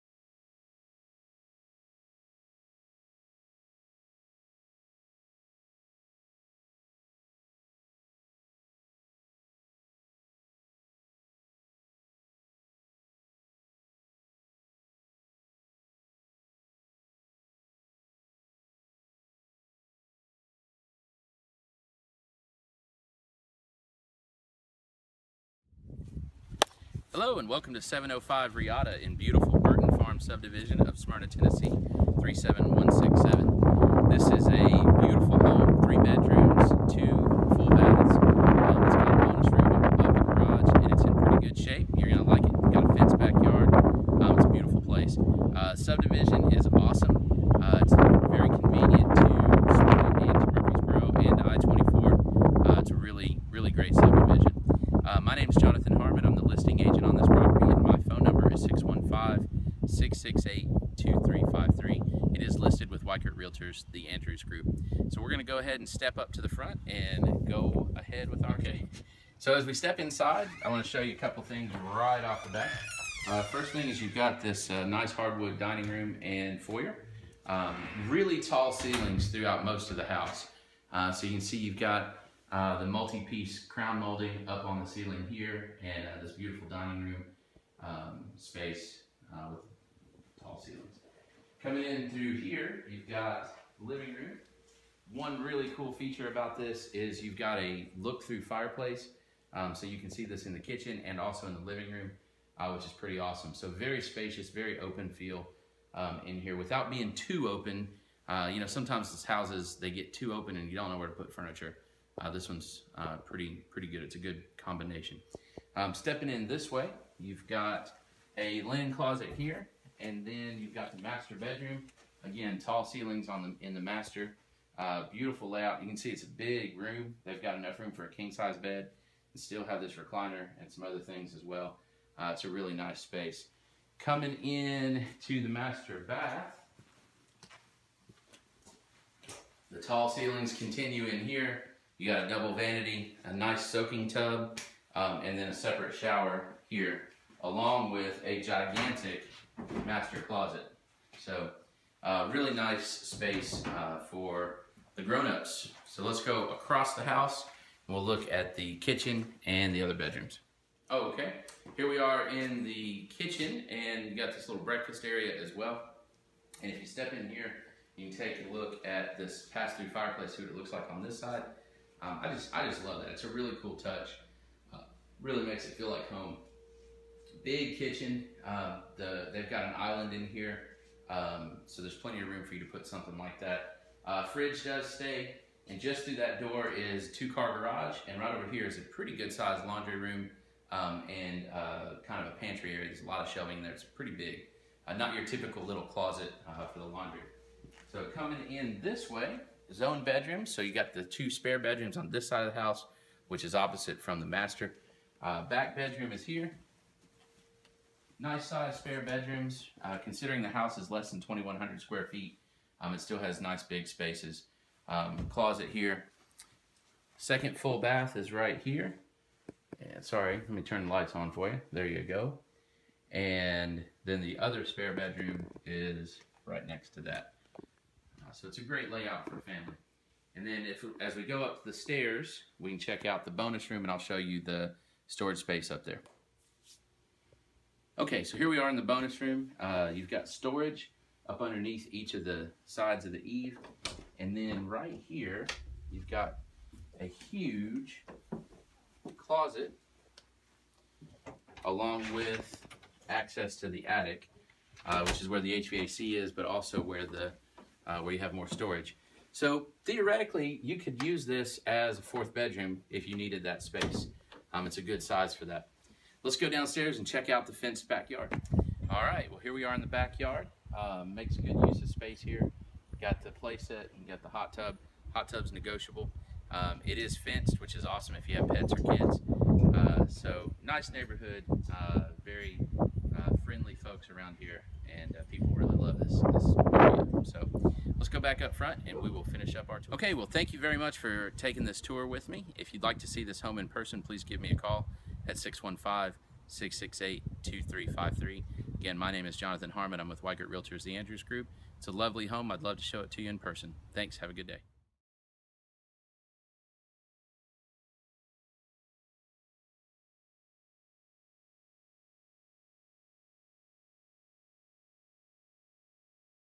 SOLD! 705 Riata Ct, Smyrna TN, Jonathan Harmon
Great family home in beautiful neighborhood! Nestled on the edge of Smyrna and Murfreesboro! Large bedrooms and spacious bonus!
Weichert Realtors The Andrews Group